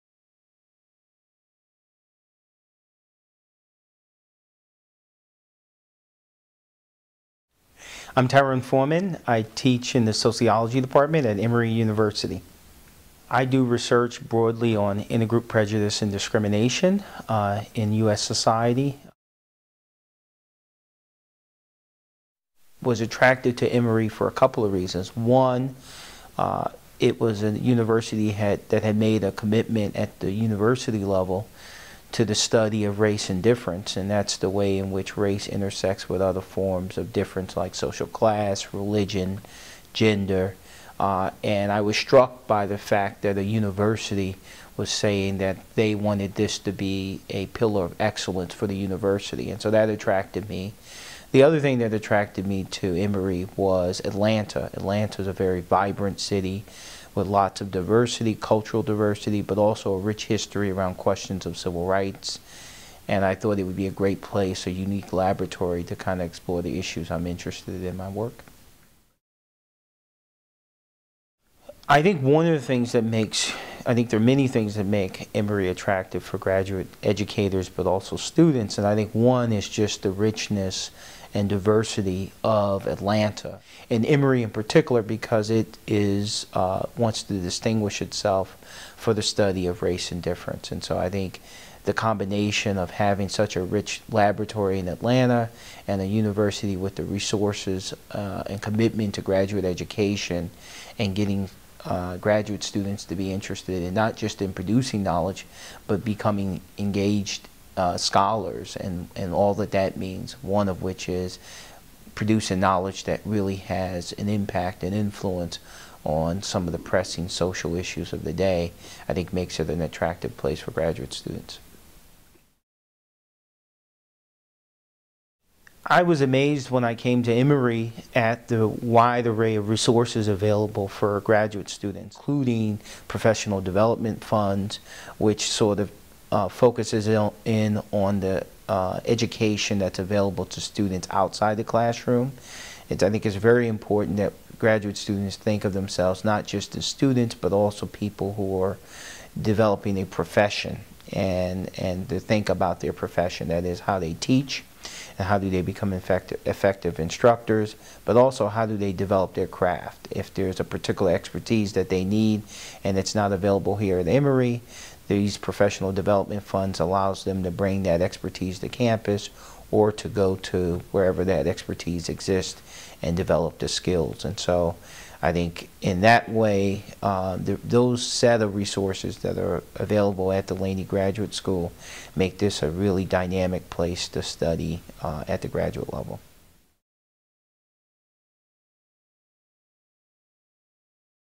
Tyrone Forman Why Emory
Emory's Tyrone Forman, Associate Professor of Sociology, discusses why he chose Emory and why Emory is an attractive place for graduate students.